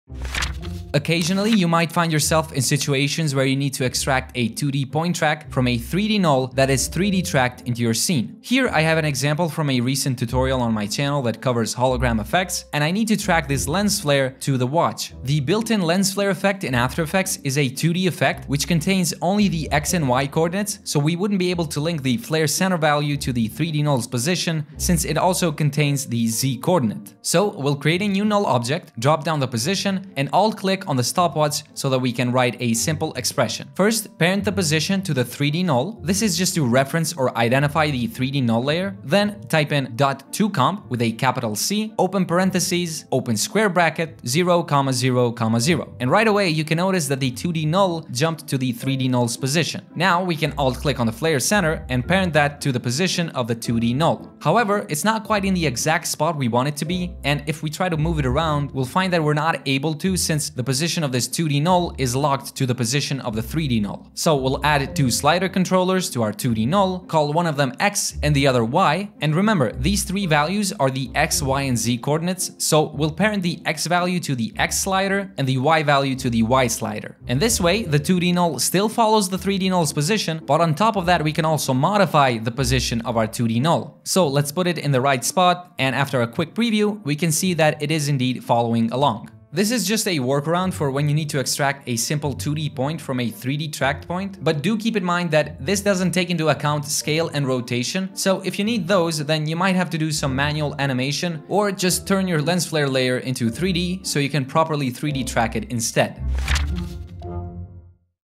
3D to 2D Track - After Effects Tutorial
How to extract a 2D Tracking Point from 3D Tracking Data.